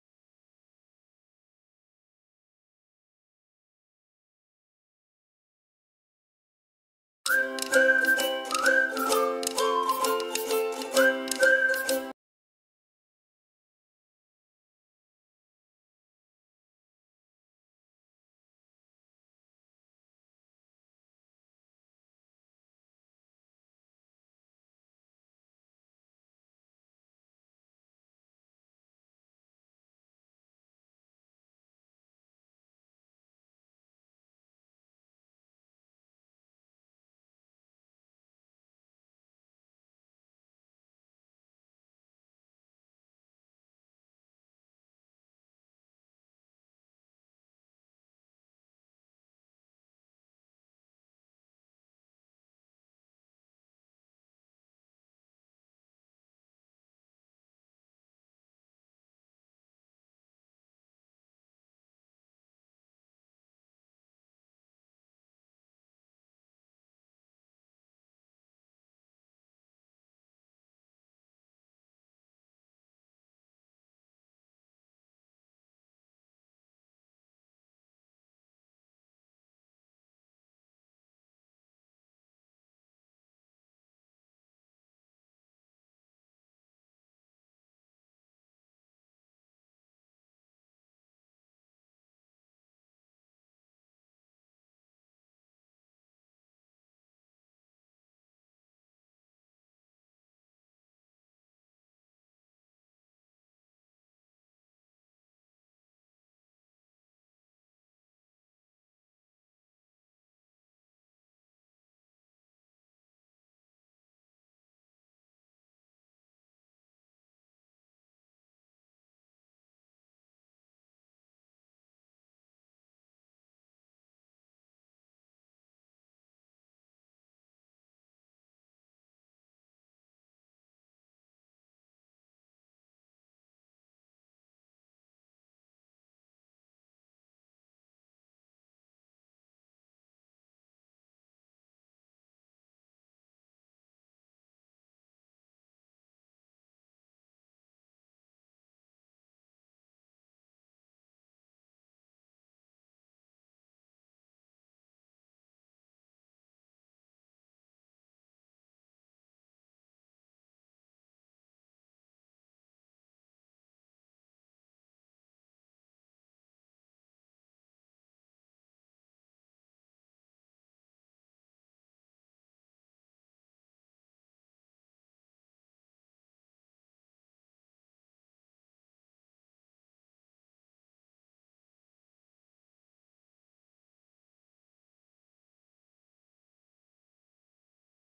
Lembrancinhas de páscoa
Nesse vídeo vamos fazer essa linda e gostosa lembrancinhas de páscoa!
Moldes de presente no link abaixo!